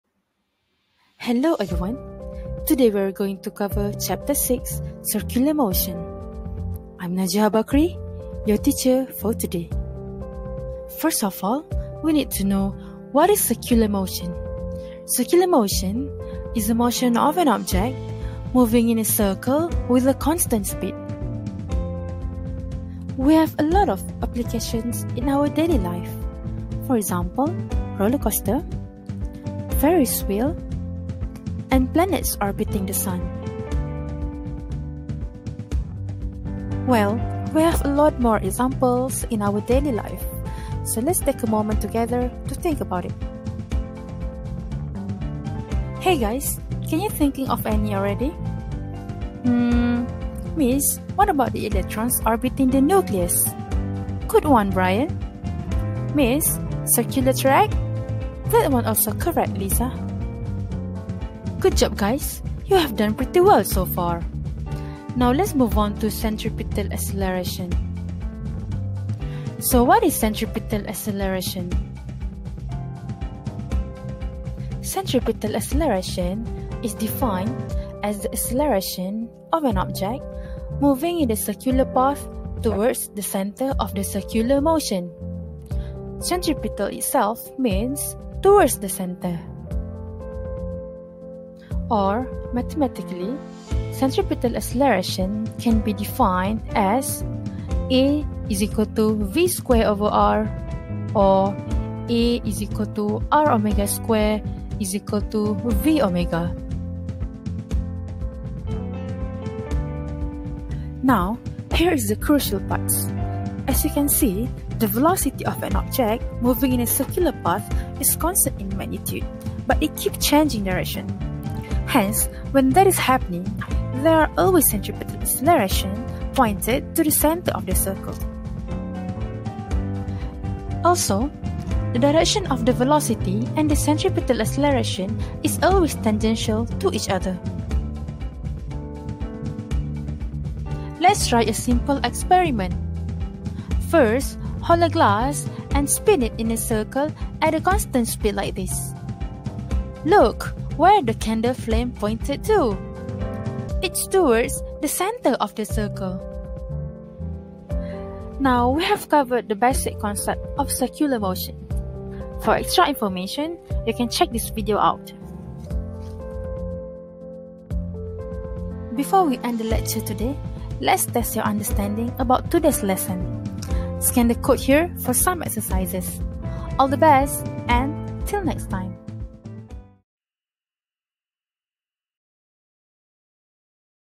(Matriculation) Chapter 6: Circular Motion
[Matriculation syllabus]
Physics Semester 1
Chapter 6 - Circular Motion

*we only covered the basic concept of circular motion only since lecture syllabus requires C1 and C2 level of difficulties*